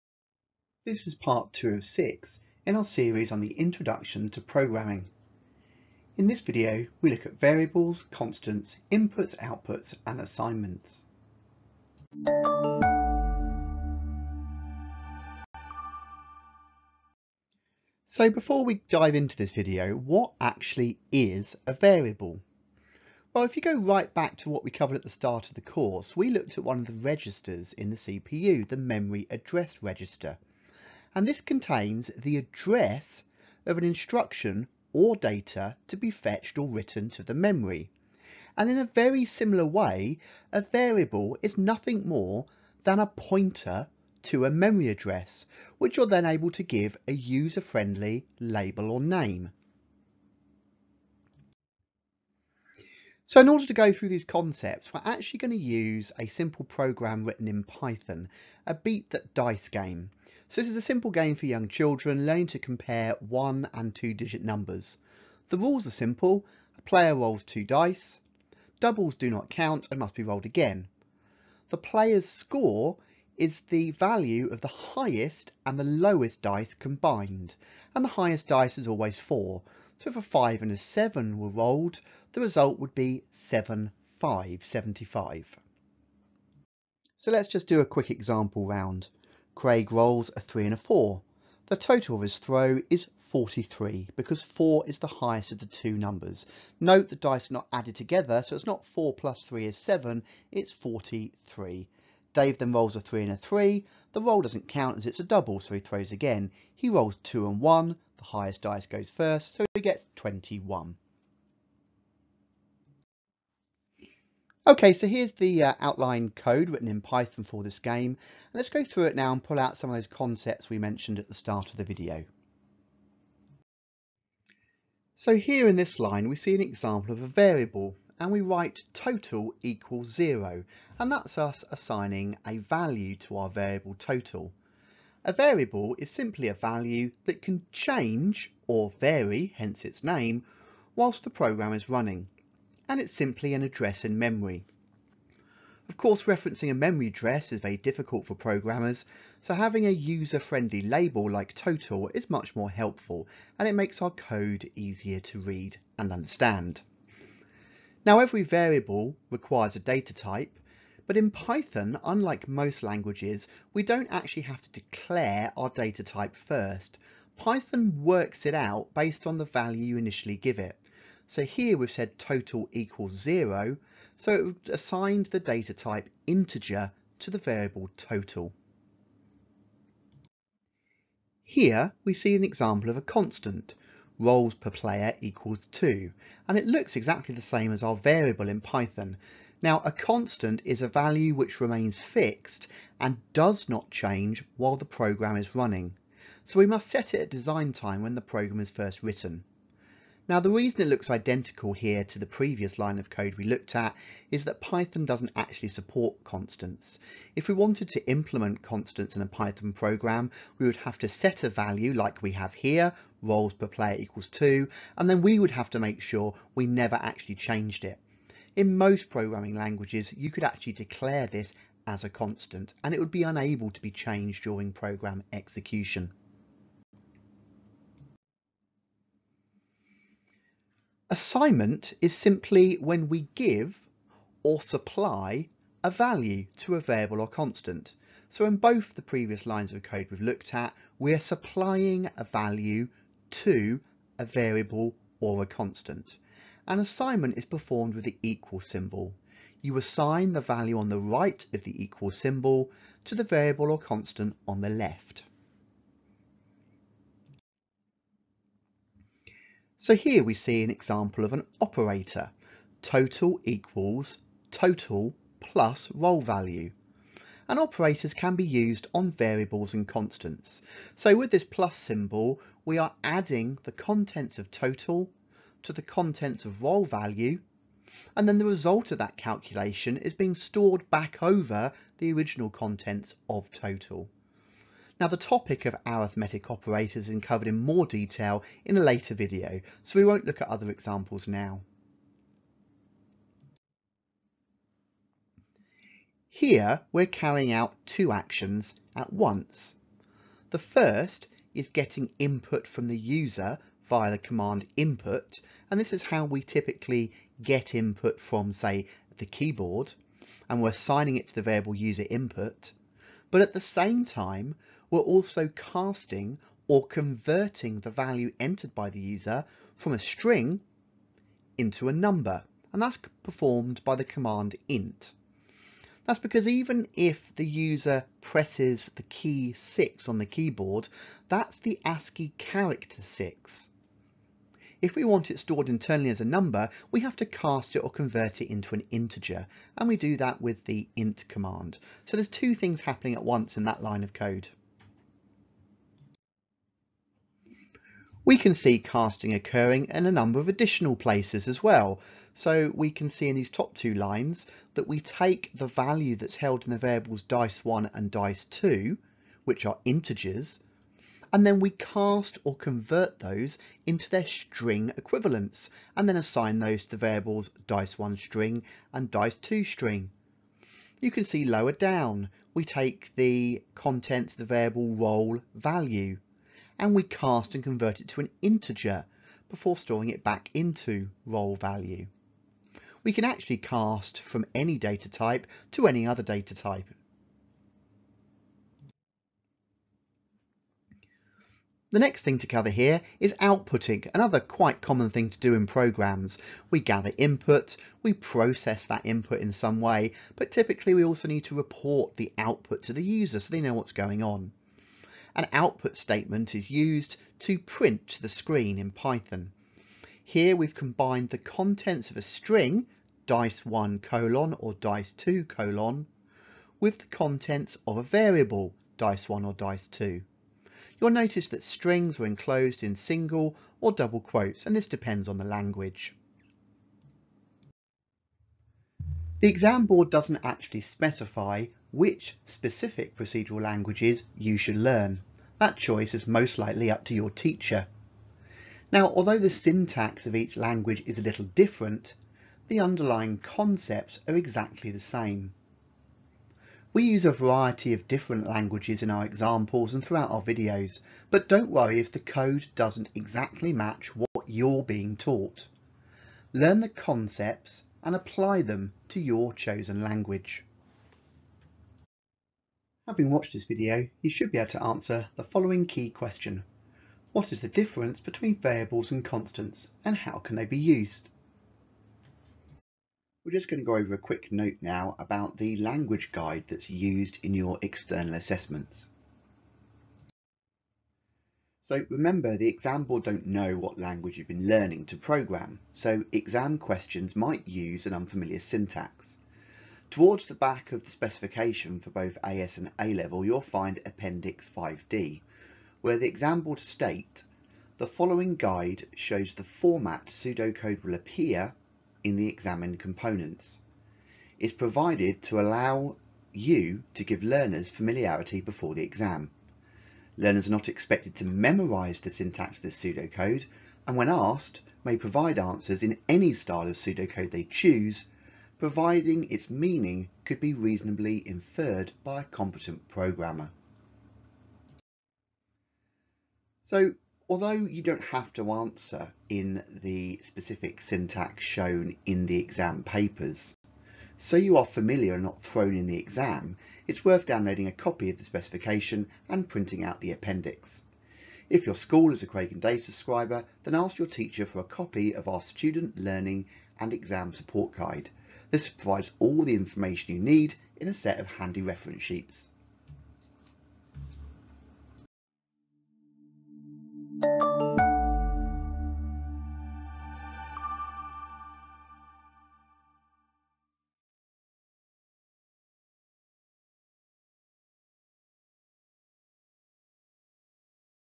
41. OCR A Level (H046-H446) SLR8 - 1.2 Introduction to programming part 2 variables & constants
AS Level 1.2.3a
A Level 1.2.3a

00:00 Intro
00:17 Variables and Constants: What is a Variable?
00:52 Beat That Dice
07:08 Different Procedural Languages
07:42 Key Question
07:53 Languages Guide for Use in External Assessments
08:00 A Note About Pseudocode in Your Exams
09:15 Outro